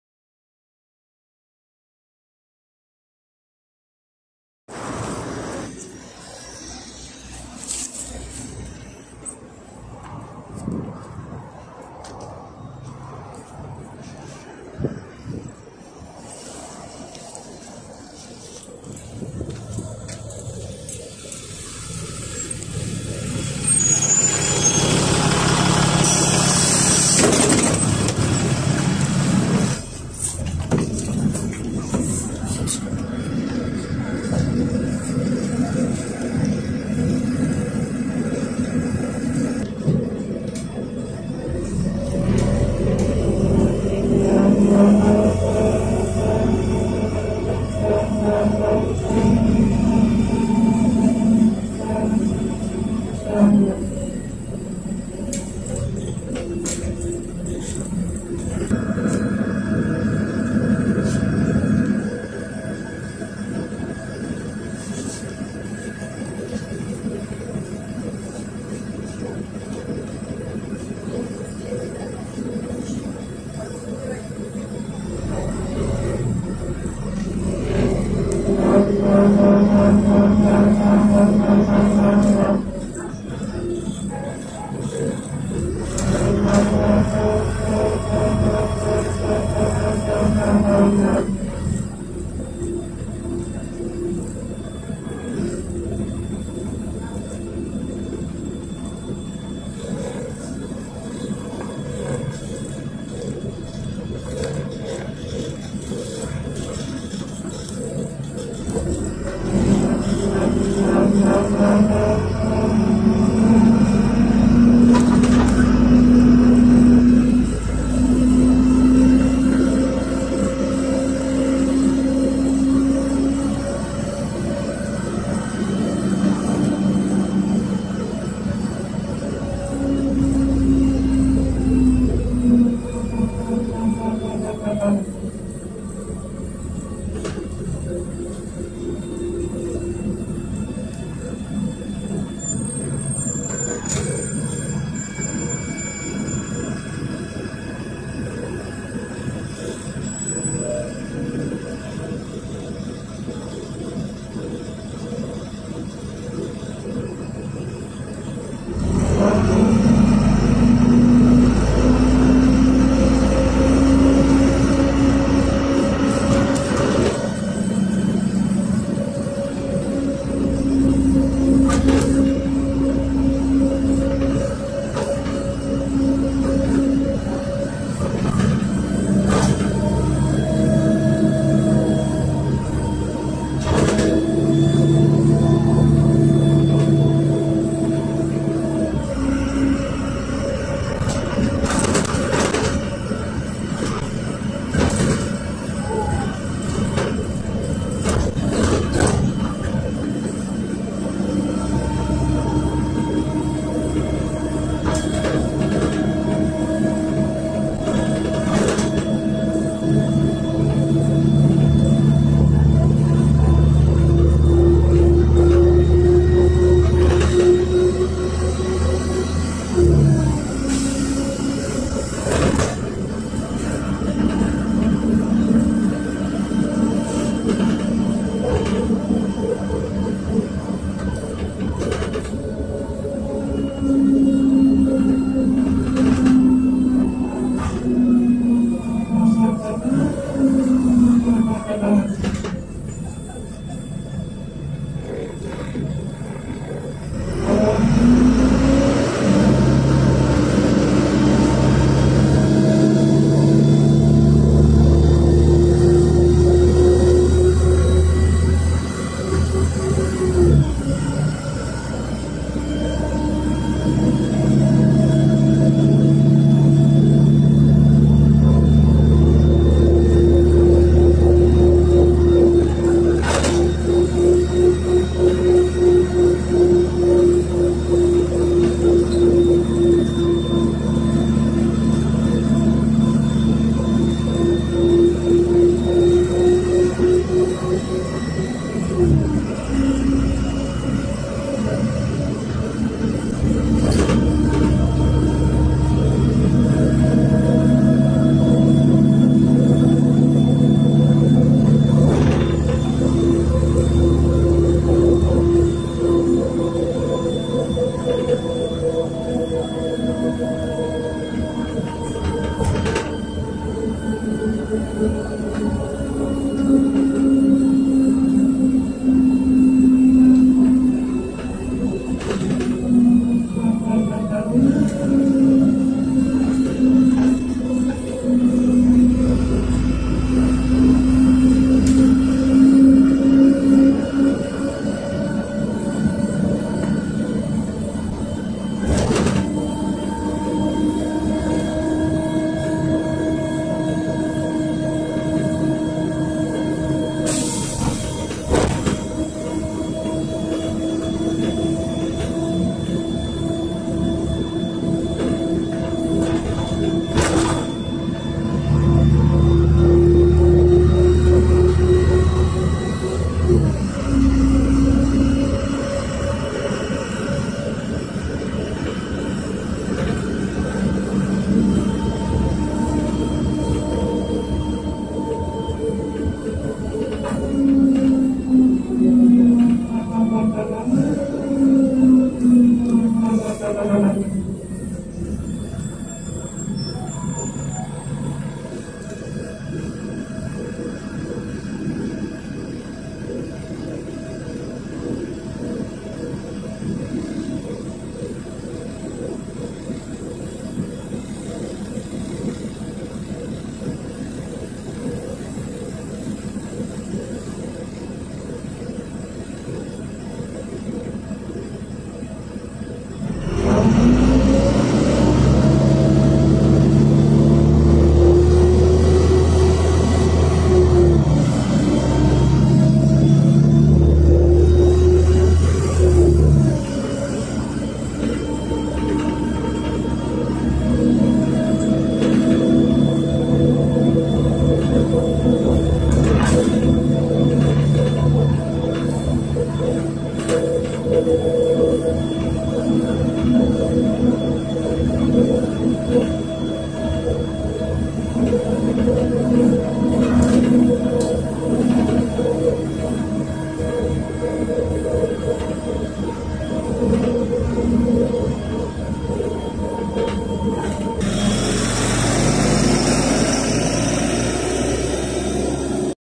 поездка на автобусе НЕФАЗ 5299 30-22 ( 2008 г.в ), у459хр174, маршрут 37с
поездка на автобусе НЕФАЗ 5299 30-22 маршрут№37с ж/д вокзал сад Исаковский

предприятие ИП Осинский гк кумир-автолайн

гос номер у459хр174
кол-во единиц данной модели ( всего/работают ) 7/7
стоимость проезда 30 рублей по транспортной льготы 12.50 по безналу 26 рублей

завод изготовитель Нефтекамский автозавод

экологический стандарт Euro-3
двигатель Cummins 6lSBe 270B
коробка передач Voith DlWA D 854.3
кол-во мест общее/для сидения 97 ( 25+1 )
год выпуска 2008
было снято в 10.05.23